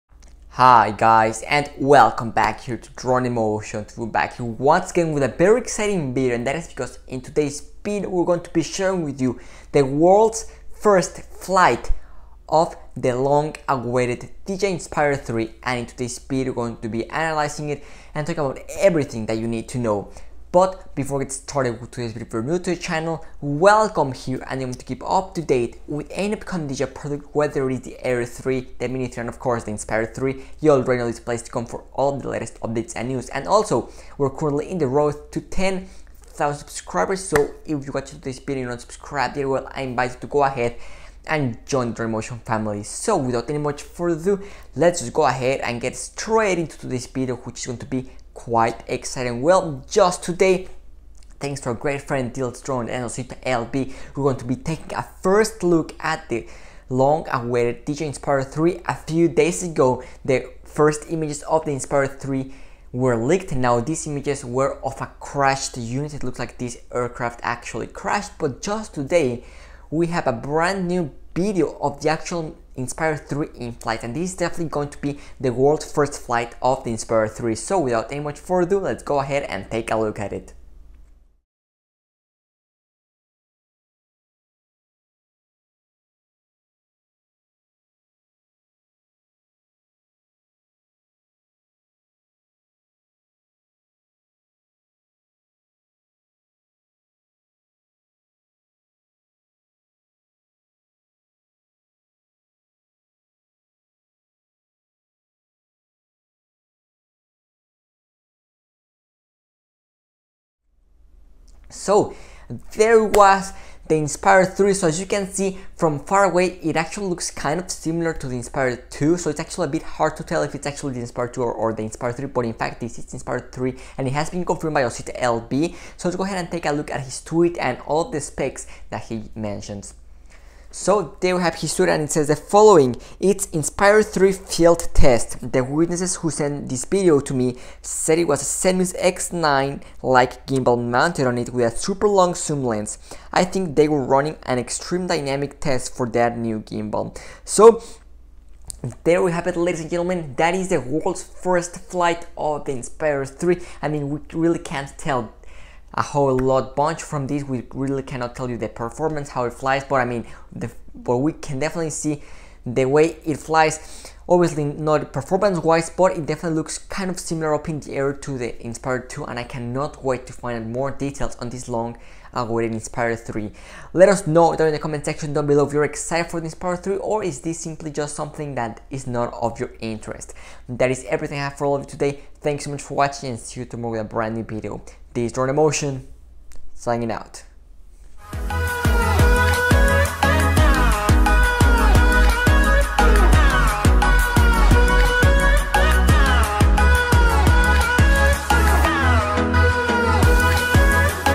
DJI Inspire 3 World’s First Flight
Today we take a look at the first ever flight of the Inspire 3.